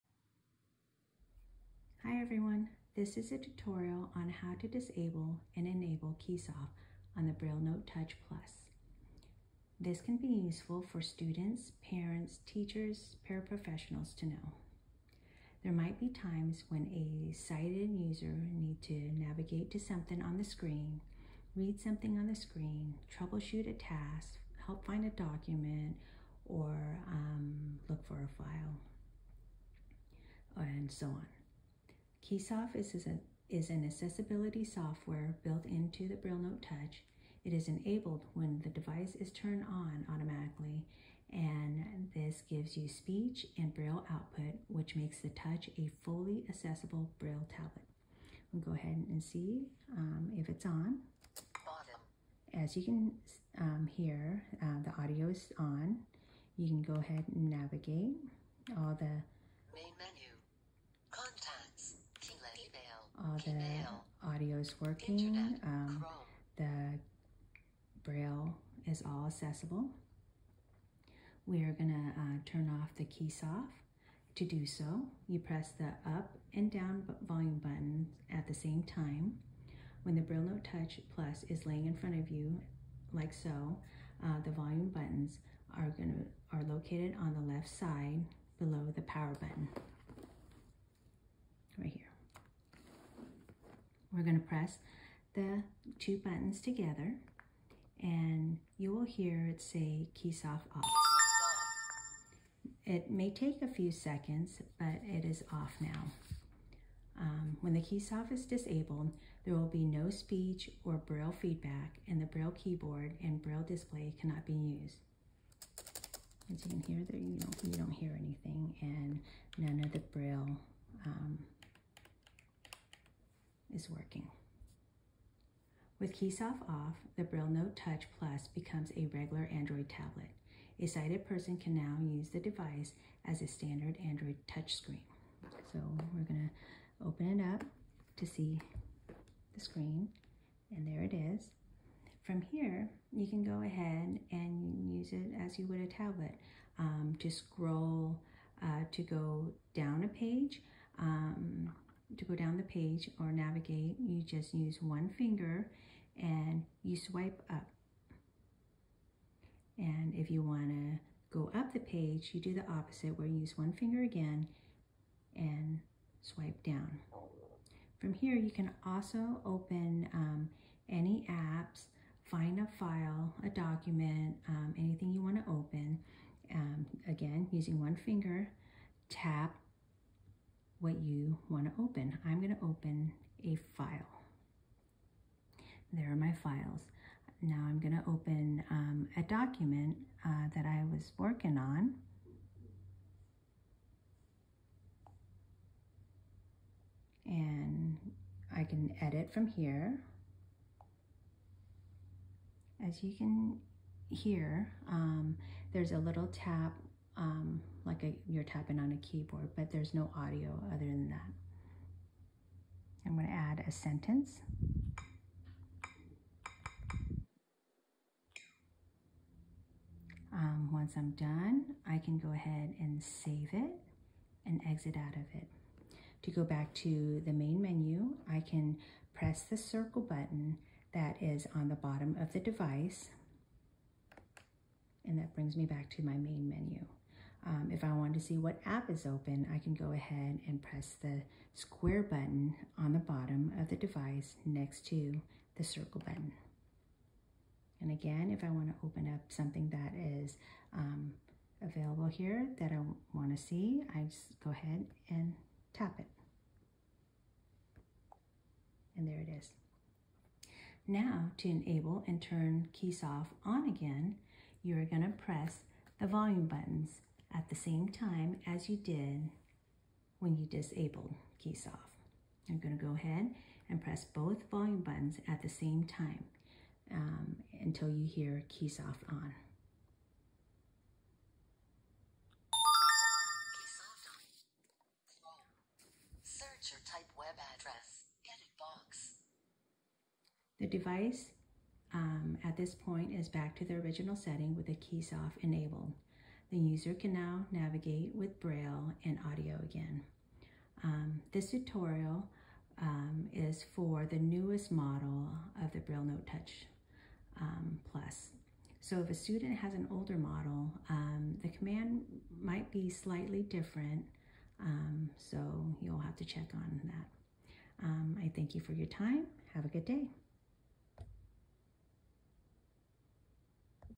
Video Tutorial: how to disable and enable KeySoft on the BrailleNote Touch Plus
This how-to video is geared toward students, parents, teachers, paraprofessionals, IT,  and anyone that will be supporting the student who uses the BrailleNote Touch Plus.  An example in the tutorial will show you what you can do when KeySoft is off.  A sighted user does not need to know how KeySoft works on the device or have a lot of experience to help the user.

Why would you turn KeySoft off?  As an IT person, you may need to help set up the device for the student.  Parents, teachers, or paraprofessionals may want to check on the student's document or help the student navigate the internet.  A new user may need assistance with finding a file.  These are just some examples of why KeySoft would be disabled.

1.  To disable KeySoft - press both volume buttons at the same time.  You will hear "KeySoft off".  KeySoft is now disabled.
2.  To enable KeySoft - press both the volumes buttons at the same time, as you did when you disable it.  You will hear "KeySoft on".  KeySoft is now enabled.